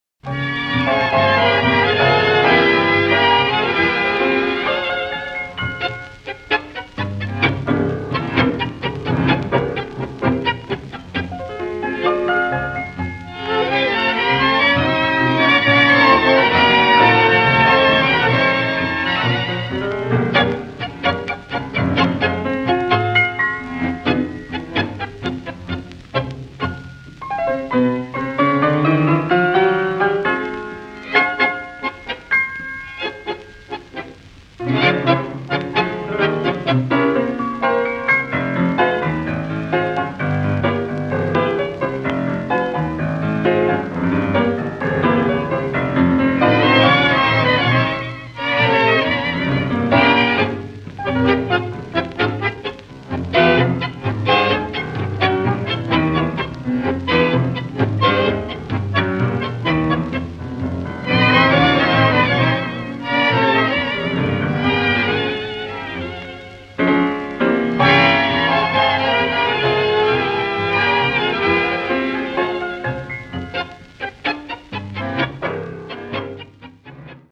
Argentine Tango dance technique 7: Walking forward and backward
Use this element to change your walking from forward to backward and vice versa.